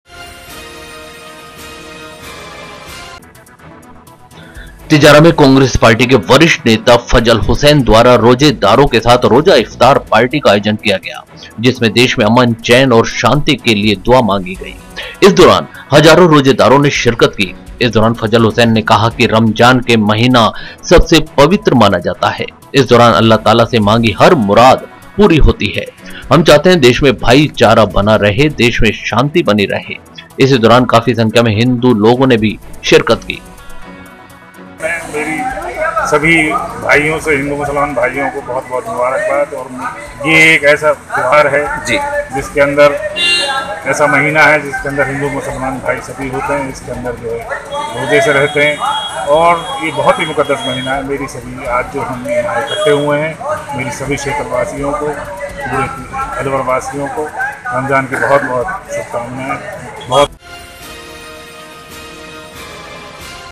तिजारा में हुआ रोज़ा इफ्तार पार्टी का आयोजन, कांग्रेस वरिष्ठ नेता फज़ल हुसैन भी रहे शामिल, देश में भाई
-तिजारा में कोंग्रेस पार्टी के वरिष्ठ नेता फजल हुसैन द्वारा रोज़ेदारो के साथ रोजा इफ्तार पार्टी का आयोजन किया गया ,जिसमे  देश मे अमन  चैंन ओर शांति के लिए दुआ मांगी गई,इस दौरान हजारो रोजेदारो ने शिरकत की,इस दौरान फजल हुसैन ने कहा रमजान के महीना सबसे पवित्र माना जाता है इस दौरान अल्ला ताला से मांगी हर मुराद पूरी होती है हम चाहते देश मे भाईचारा बना रहे देश मे शांति बनी रहे,इस दौरान काफी संख्या में हिन्दू लोगो ने भी शिरकत की